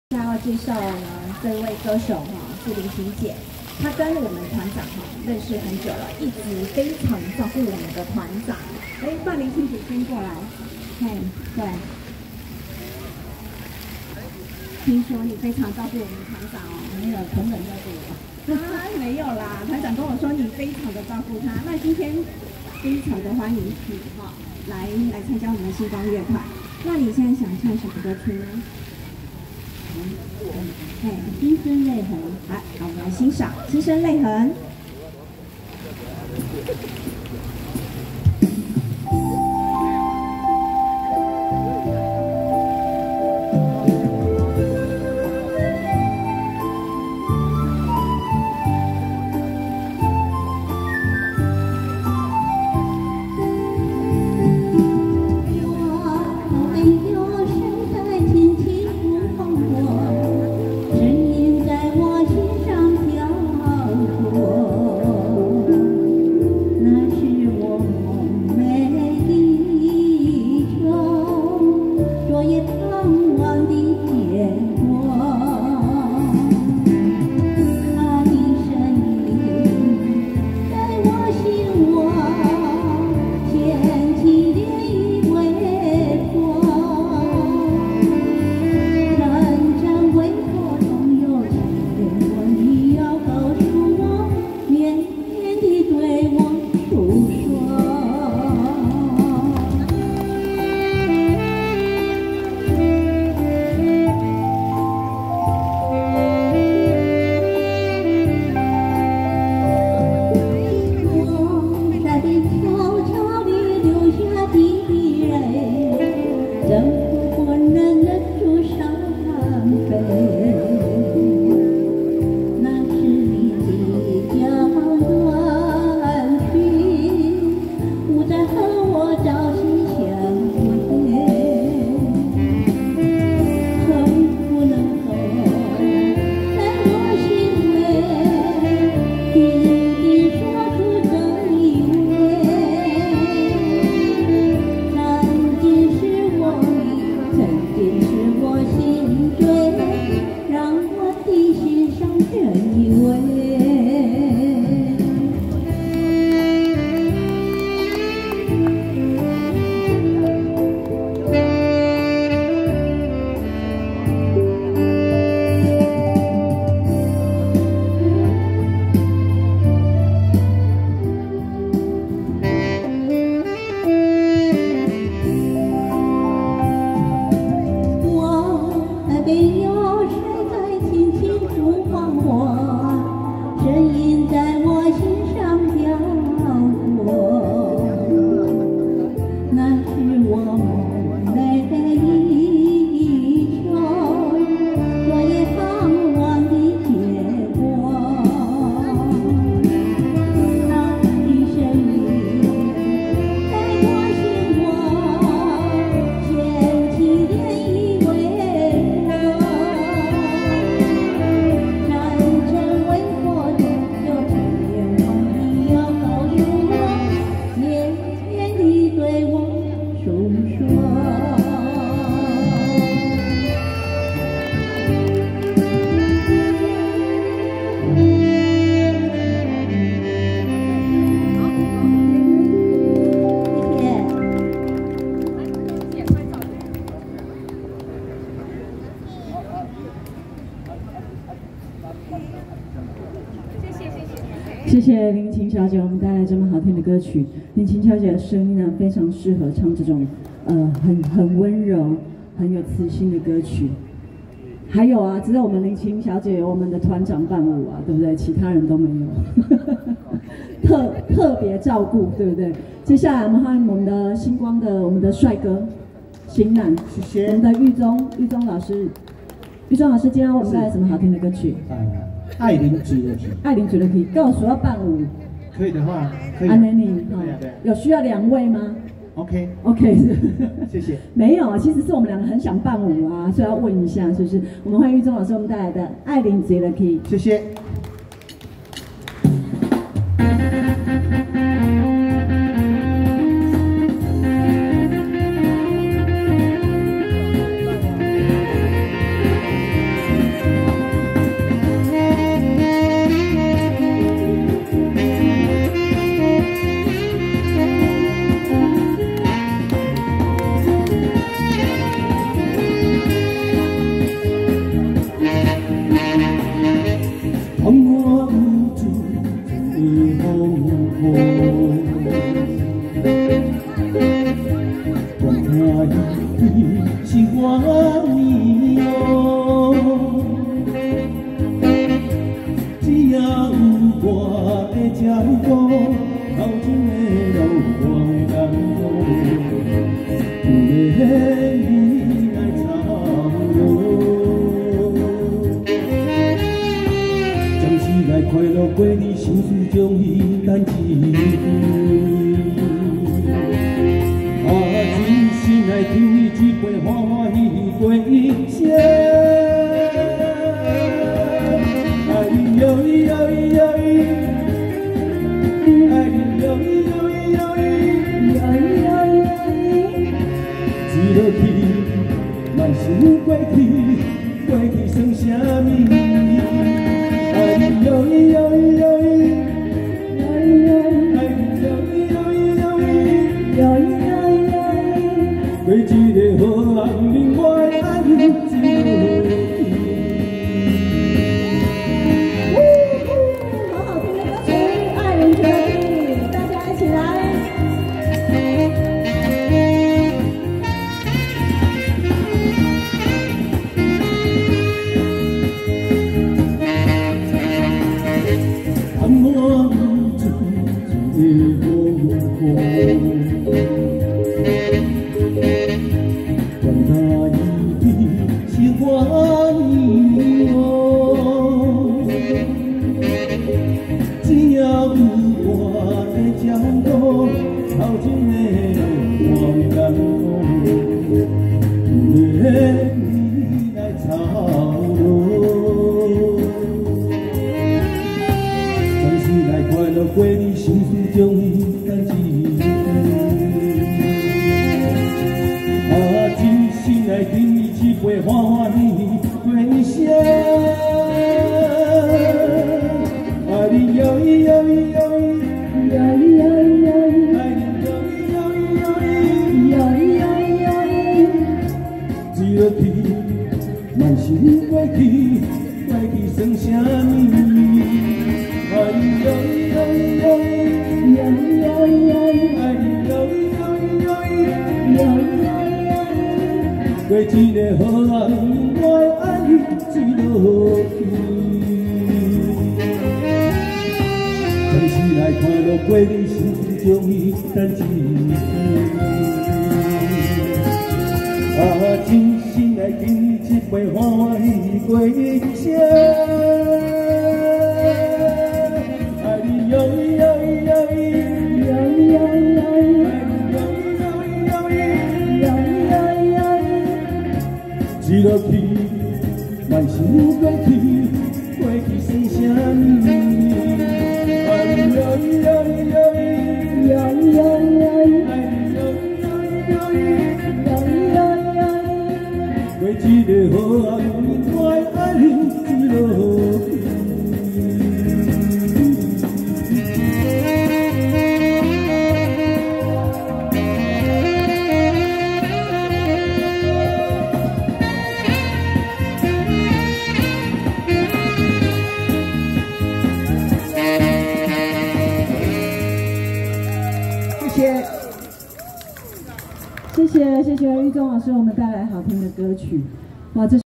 2022/12/02 市議會廣場 小週末夜演唱(8)
2022/12/02 市議會廣場 小週末夜演唱(8)

2022/12/02 20:14 攝於安平區永華路二段旁的#台南市議會廣場。
演唱樂團為#星光輕音樂。